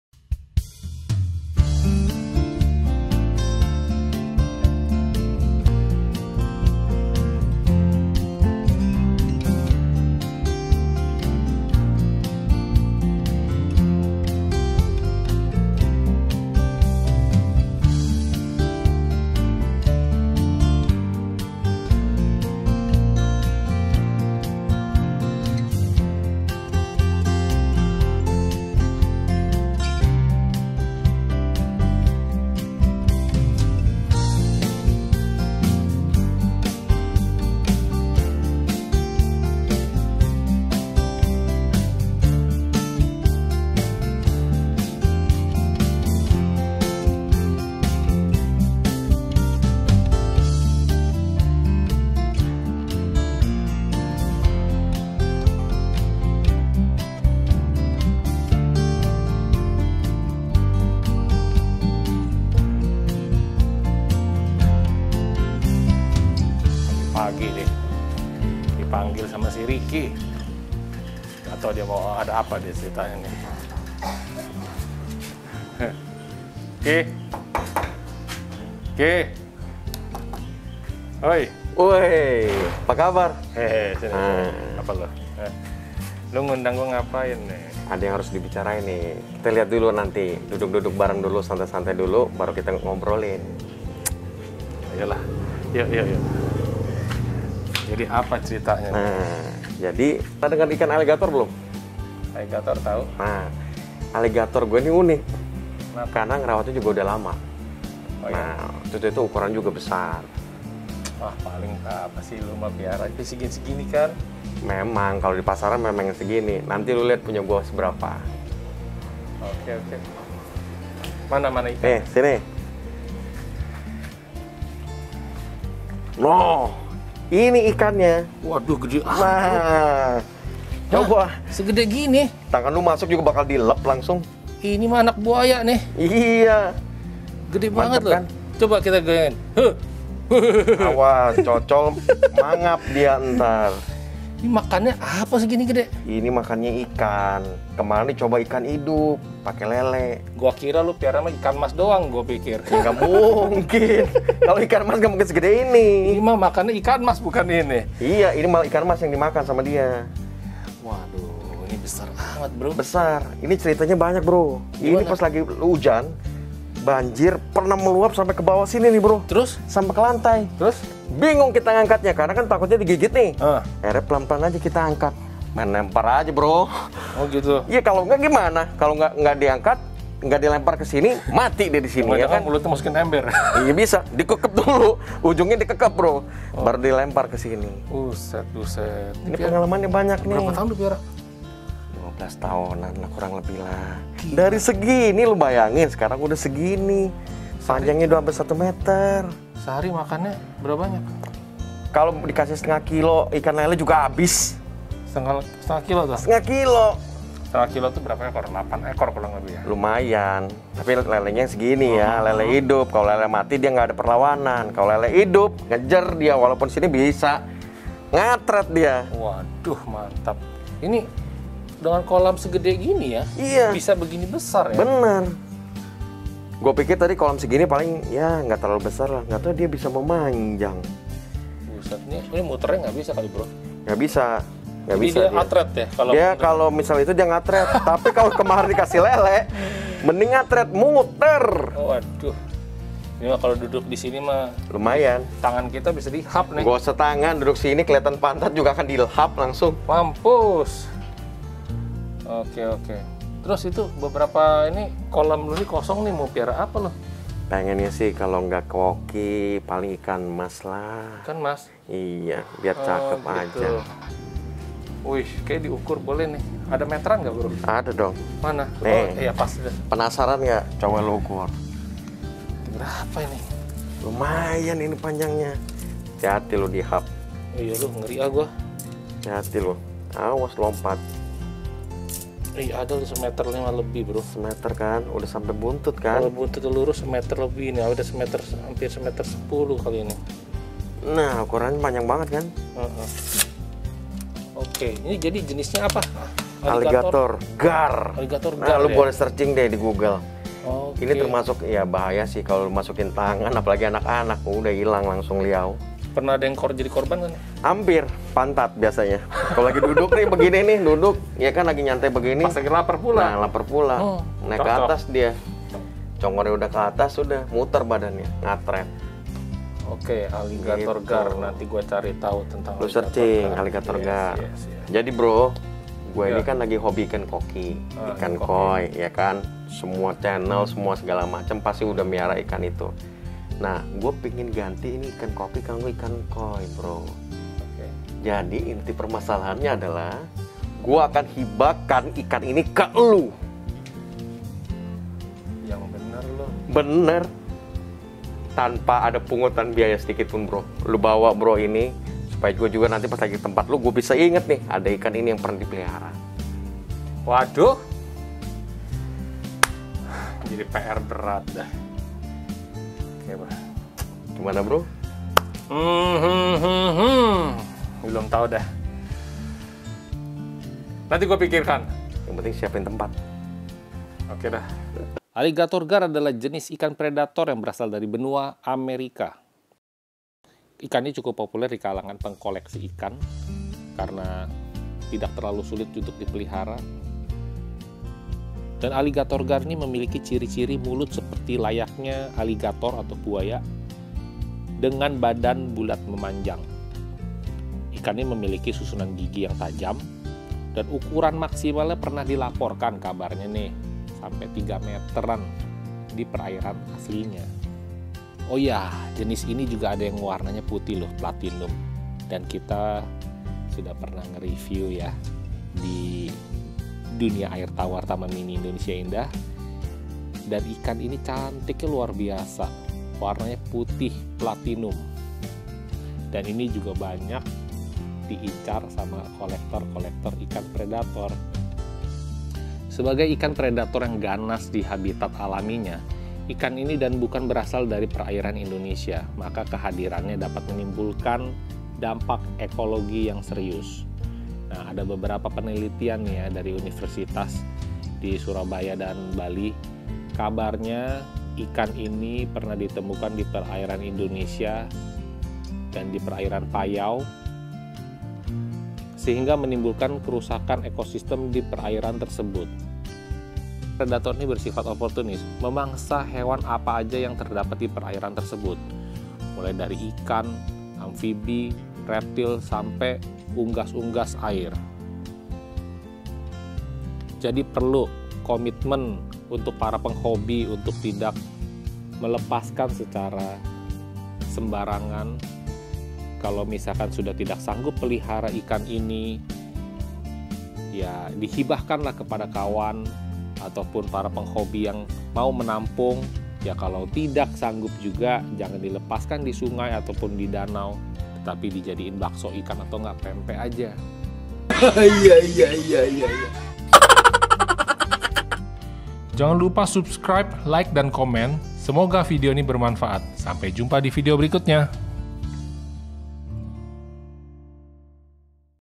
REVIEW MONSTER PREDATOR AIR 1,2 meter
Kawan kita yang satu ini ternyata punya ikan raksasa yg sdh dipelihara belasan tahun.  120cm panjang nya.  Dan keliatan nya agak kewalahan karena ukuran semakin besar.